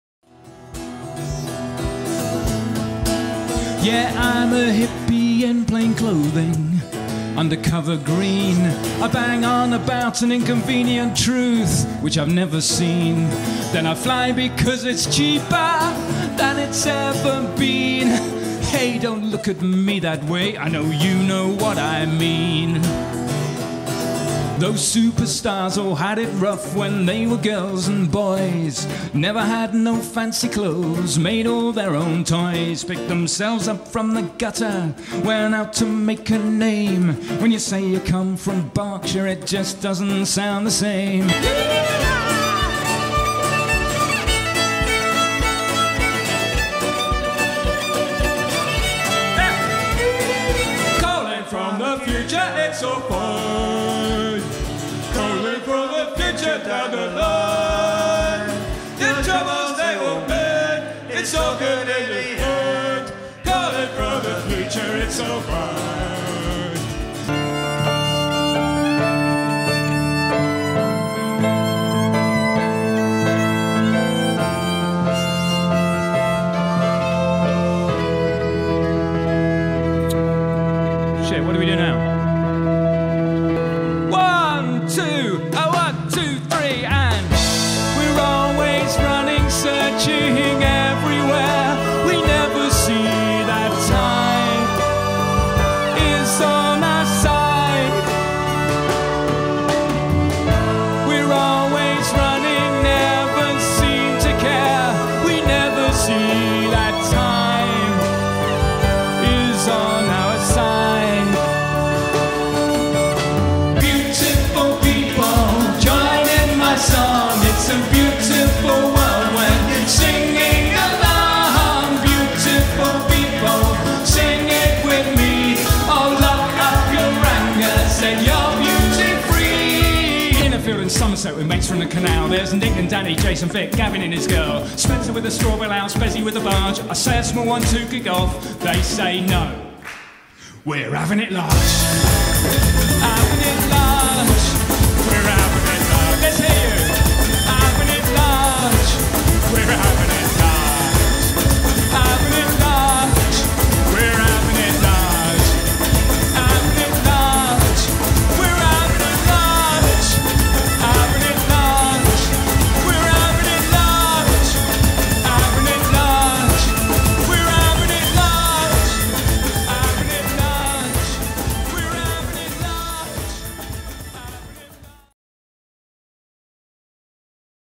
Bike To The Future Montage
A montage of highlights from a Band named Brian's bike-themed show. Featuring: Rob Whale - violin & backing vocals; Stevie Holder - bass; Florian Madigan - drums; Martin Cradick - guitar and Heather Kale, Ed Corbishley, Olli Finch, Ellie Marchant, Stephane Dougan-Hyde, Simon Kale, Jill Hollin and Sally Chown - choir.